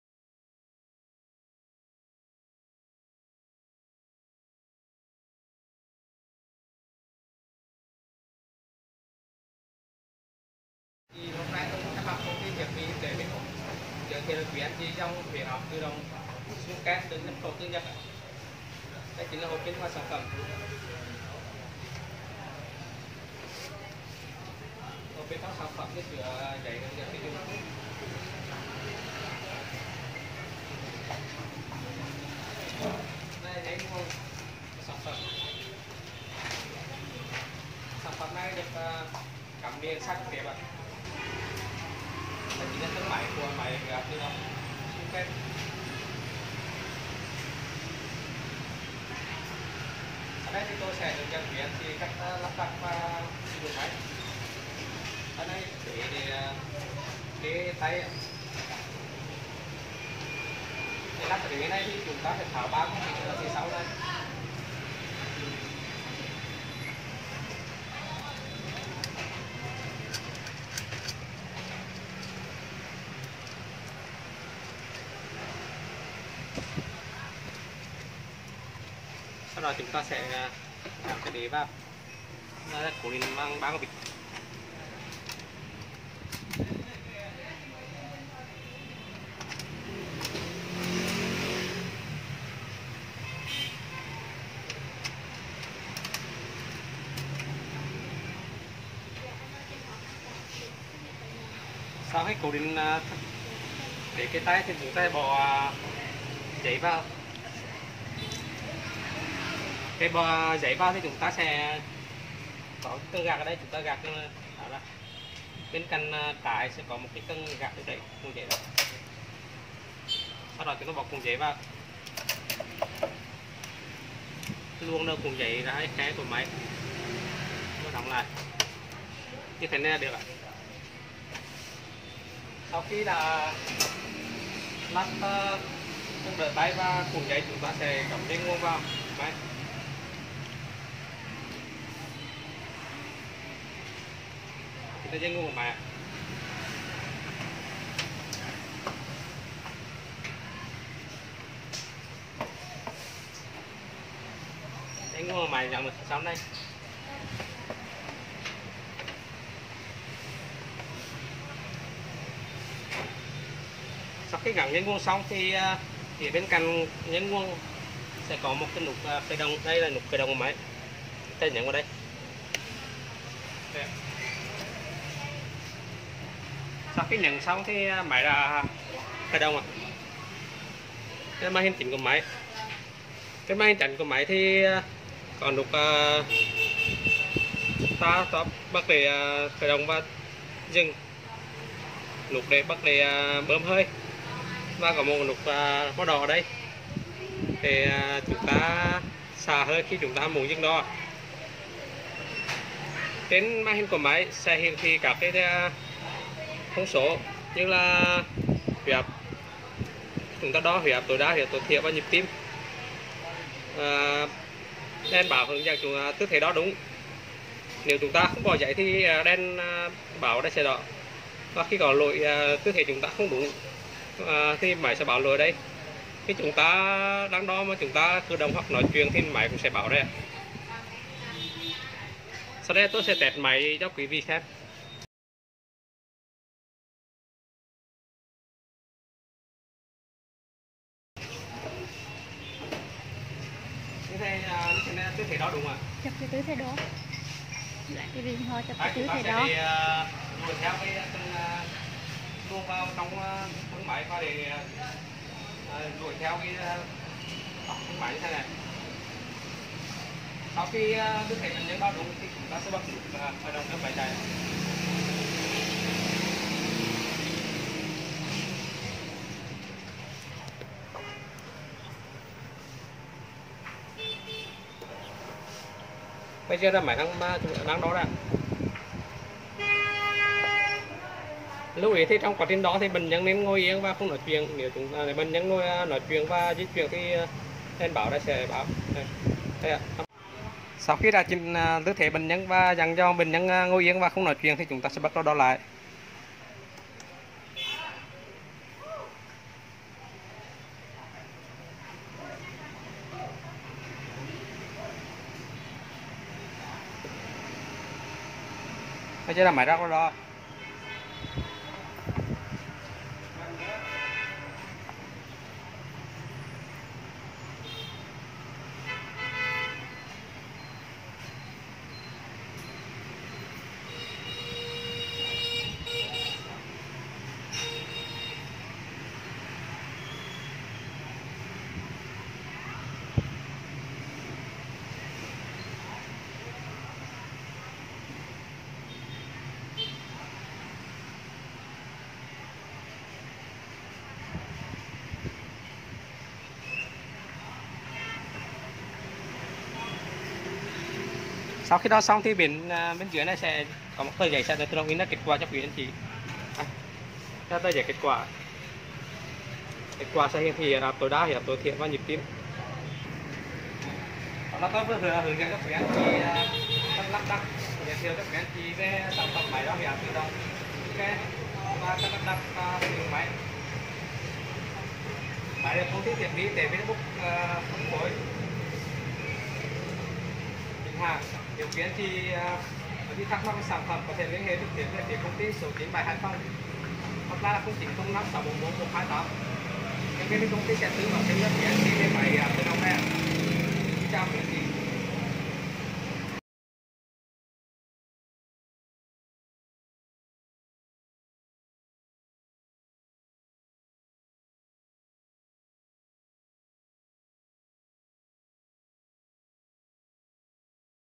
Máy huyết áp tự động Suzuken – Nhật Bản
Model: AC-05P
Hãng sản xuất: Suzuken – Nhật Bản
Nước sản xuất: Nhật Bản
AC 05P là thiết bị đo huyết áp hoàn toàn tự động cho kết quả chính xác trong 30 giây.
Nhà cung cấp thiết bị y tế Vinabook
97 Hải Phòng, Hải Châu, Đà Nẵng